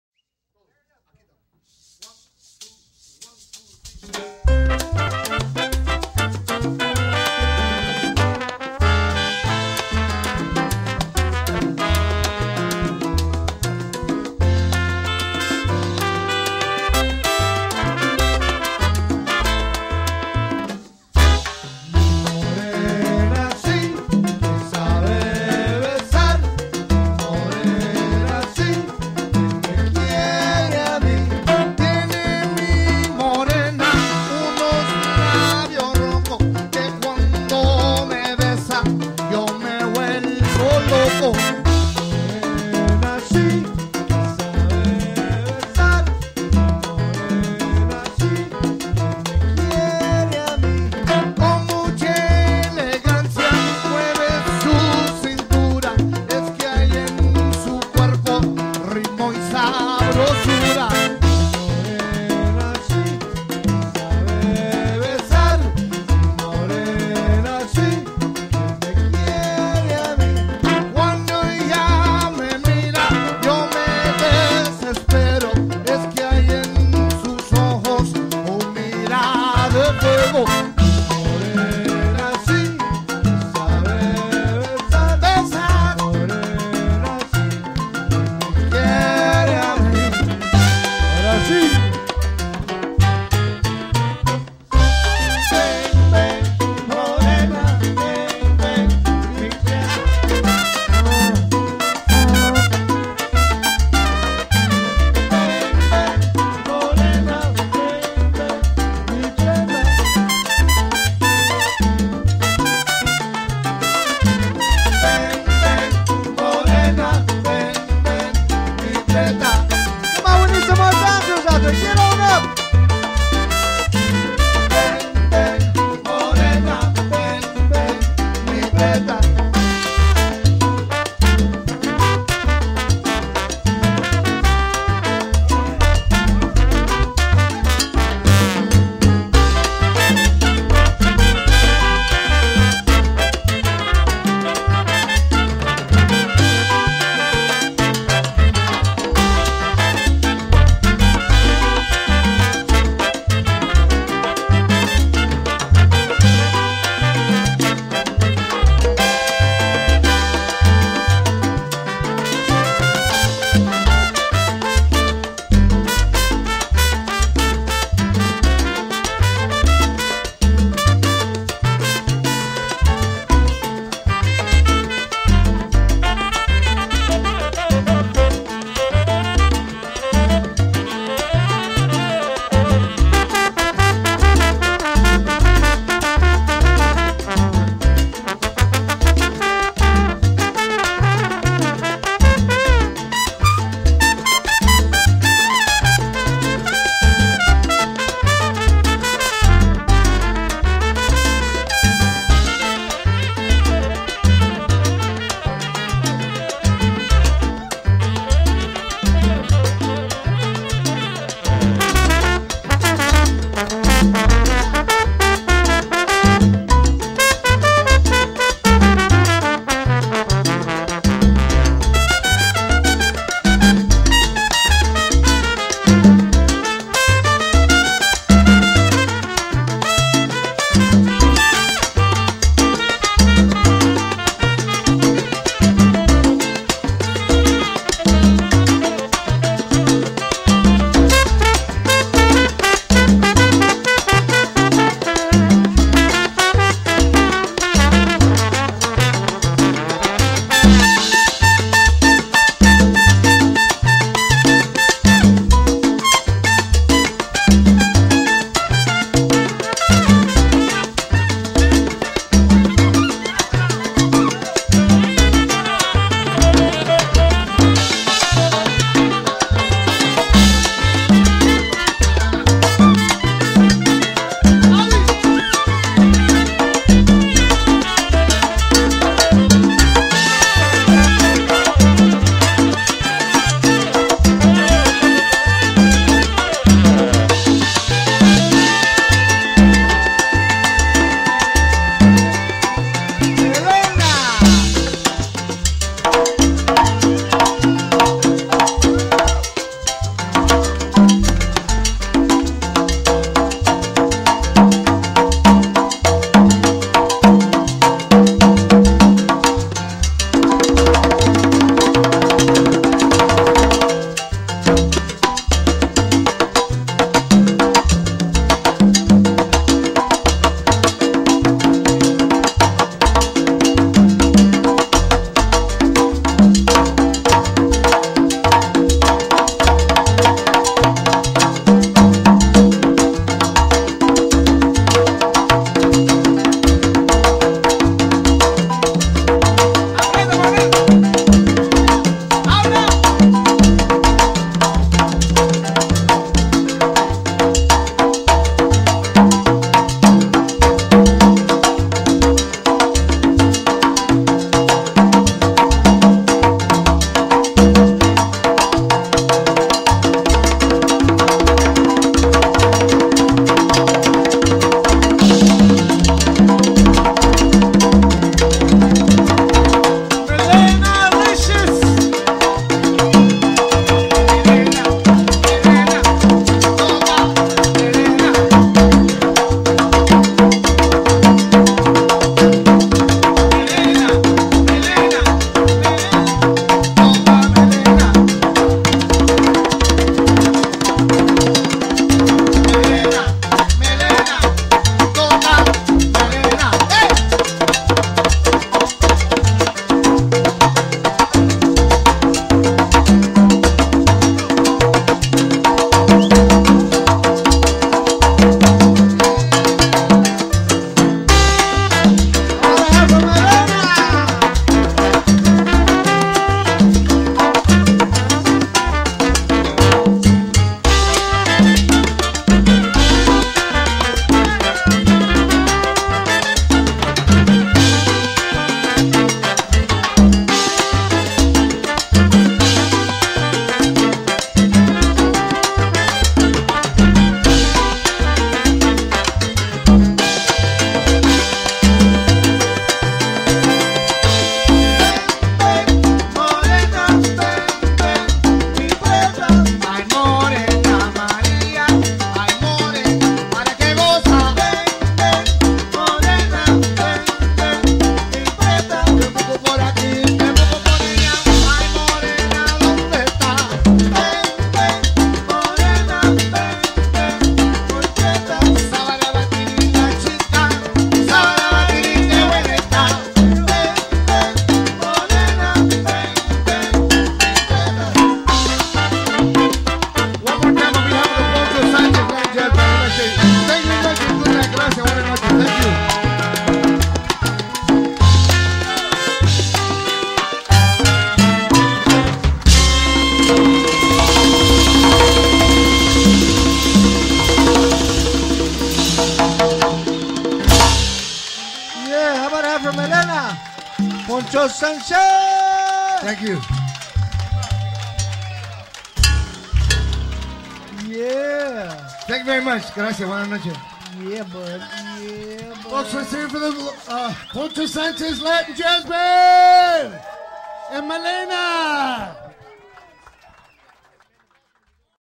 Melena at Steamers, this Sat June 27!!! RSVP!!
Melena bringing down the house performing with the Grammy Award winning Poncho Sanchez and his latin jazz band, live at Steamers Jazz Club in Fullerton, Ca. July 18, 2010. Joining Poncho is Joey DeLeon-percussion, Tony Banda-bass, David Torres-piano, Georgie Ortiz-timbales, Ron Blake-trumpet, Andy Martin-trombone and Rob Hart-sax.